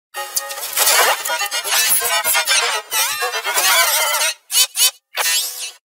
(NEW EFFECT) Klasky Csupo in TootsieSH Hodges Major (Instructions in Description)
How to Make TootsieSH Hodges Major:

1st: Gradient Map - TootsieSH Hodges Major

Spot 0: Hot Pink
Spot 1: Light Blue
Spot 2: Sandy Tan
Spot 3: R: 255, G: 192, B: 203, A: 255
Spot 4: R: 129, G: 0, B: 127, A: 255
Spot 5: Dark Blue
Spot 6: Gold

Pitches:
14
21
28
35